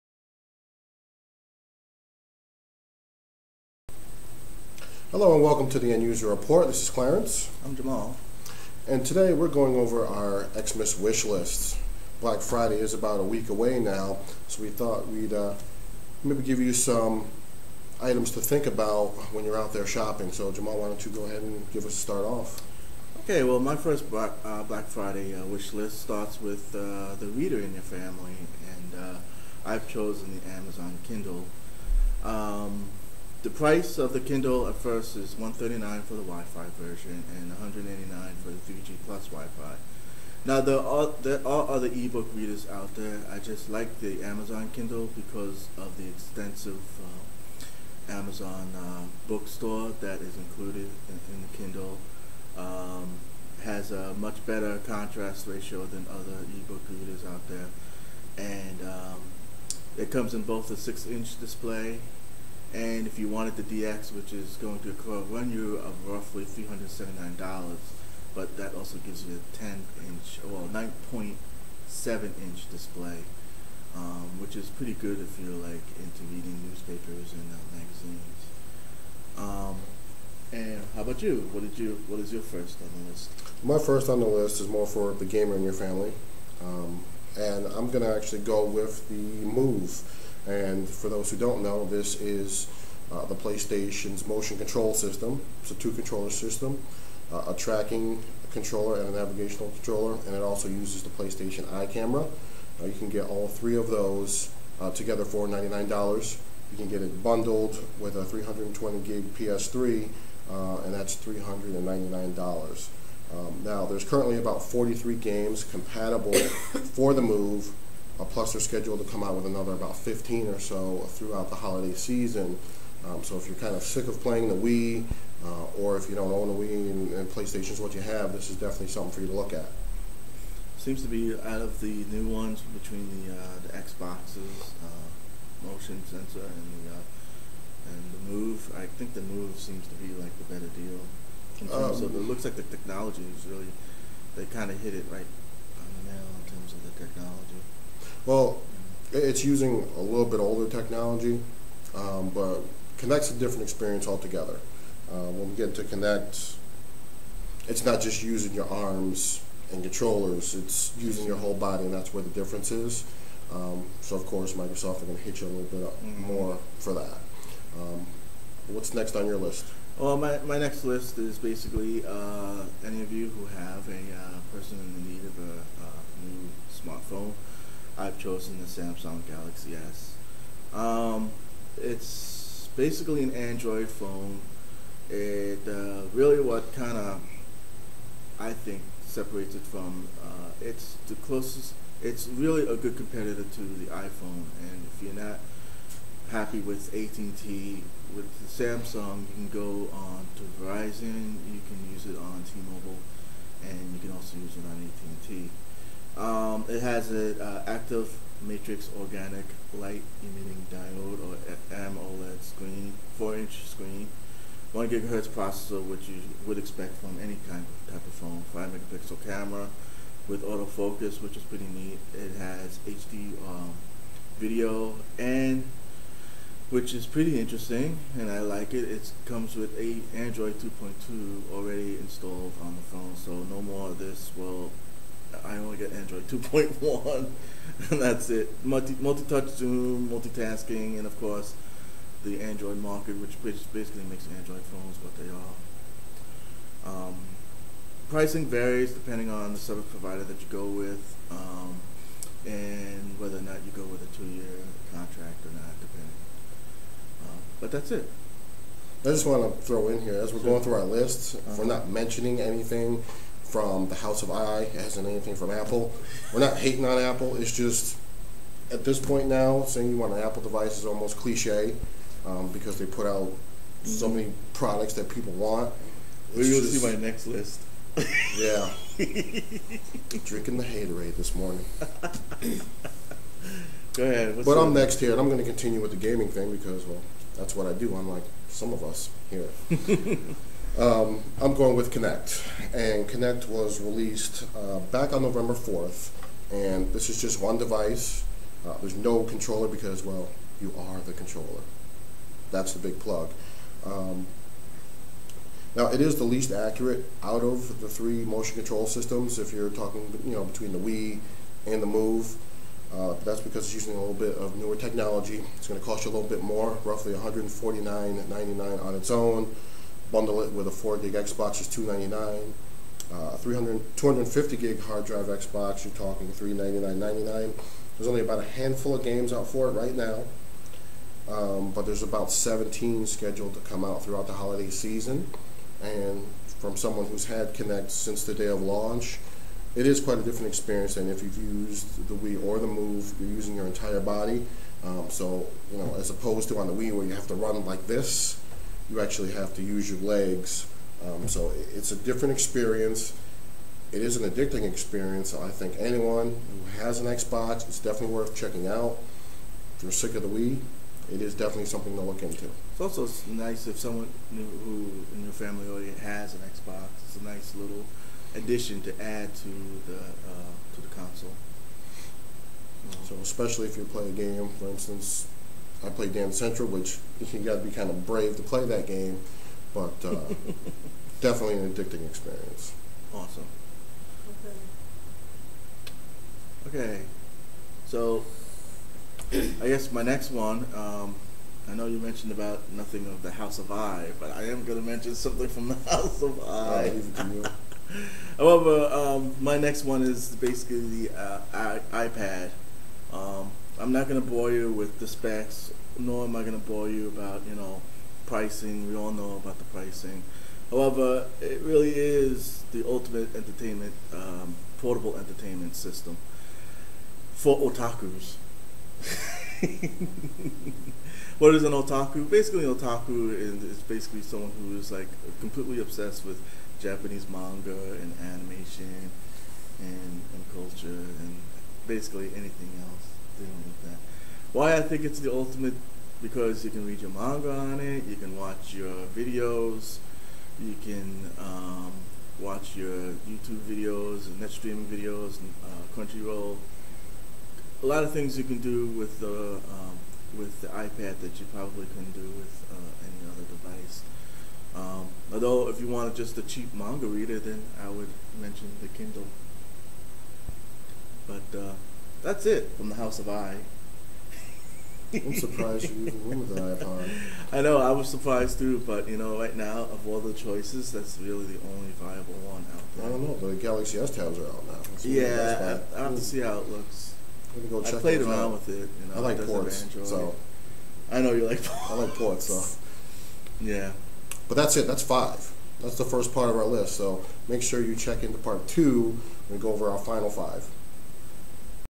Holiday shopping List 2010 1-5
This episode we talk about 10 items that you might want to pay attention to this black Friday.

These are Items 1-5:

PS3 Move: